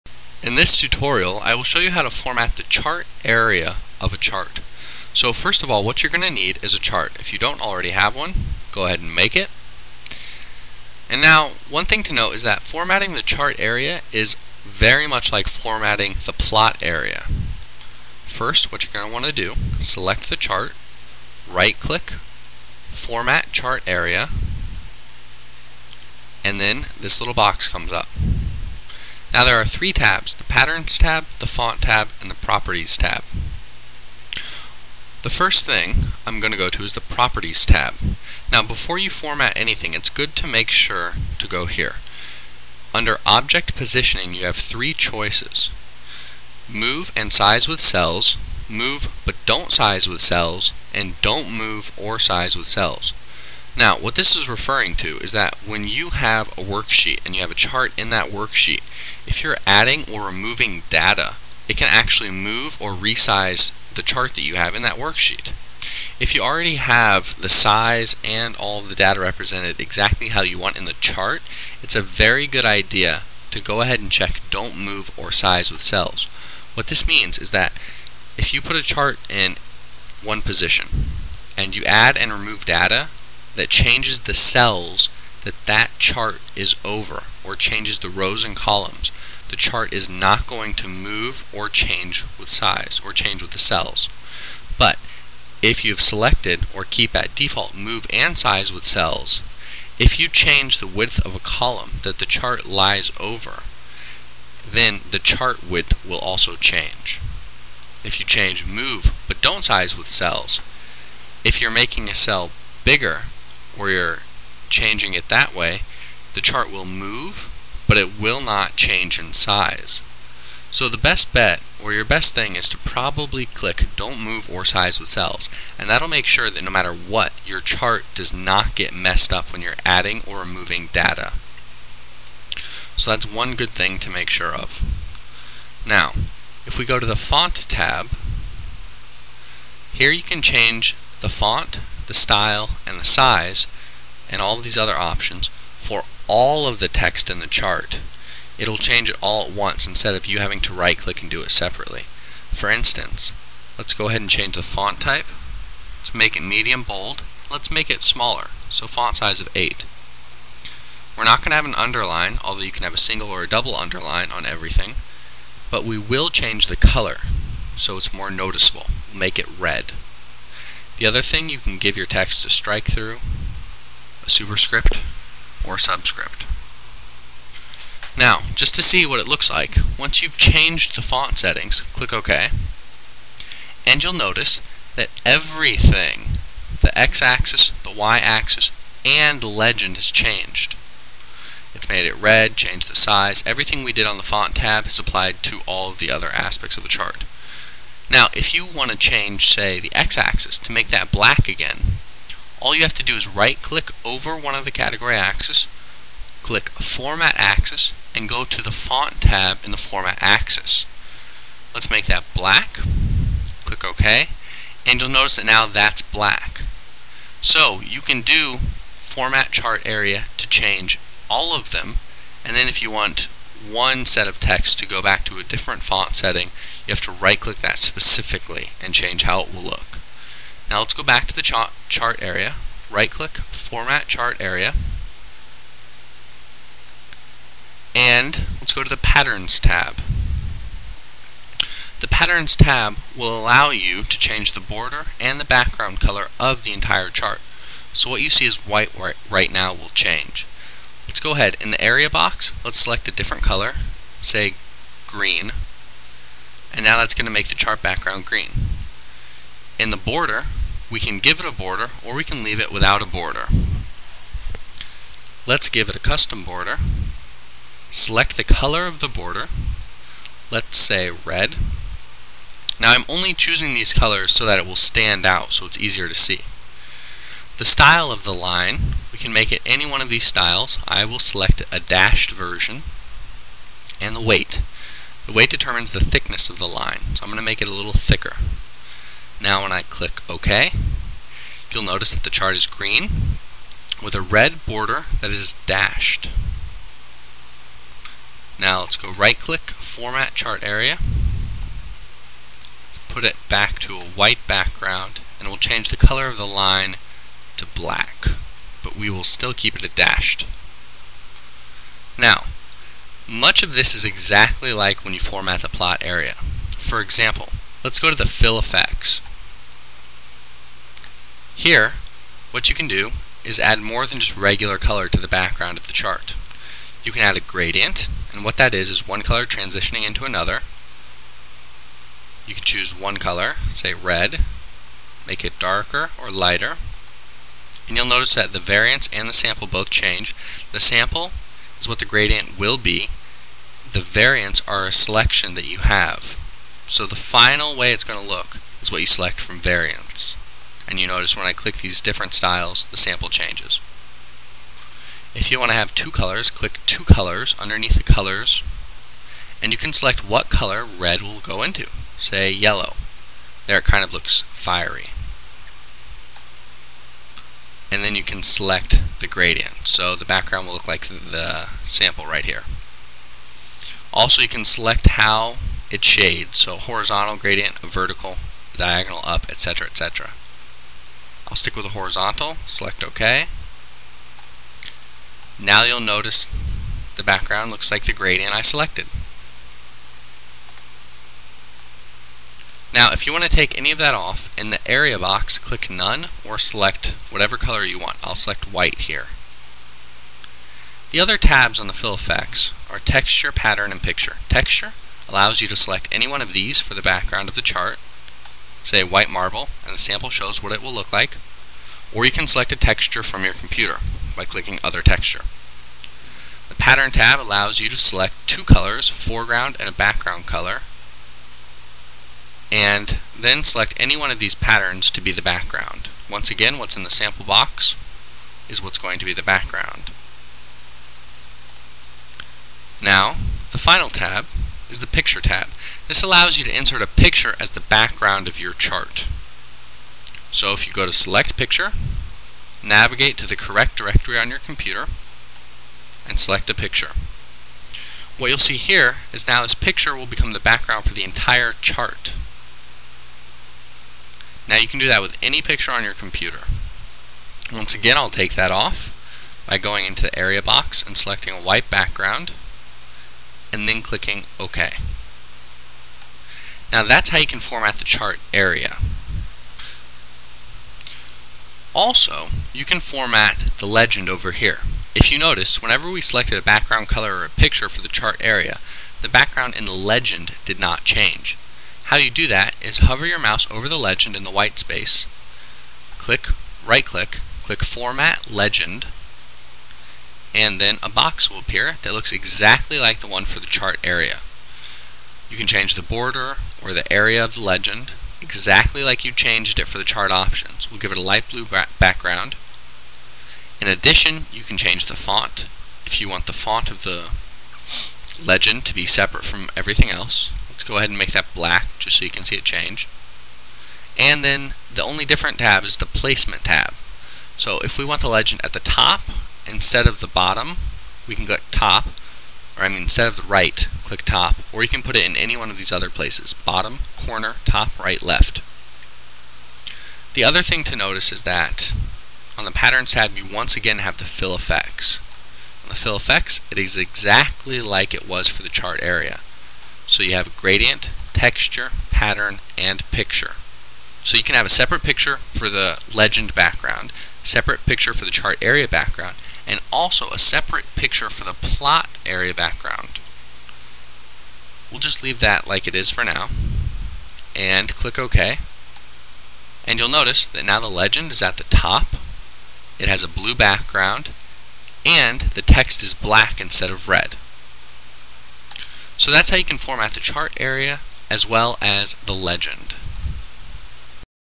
Format the Area and Legend of a Chart in Excel - Walkthrough
How to format the chart area of a chart or graph in excel. This includes adding colors, gradients, and pictures. Also, how to format the legend of a chart or graph in excel.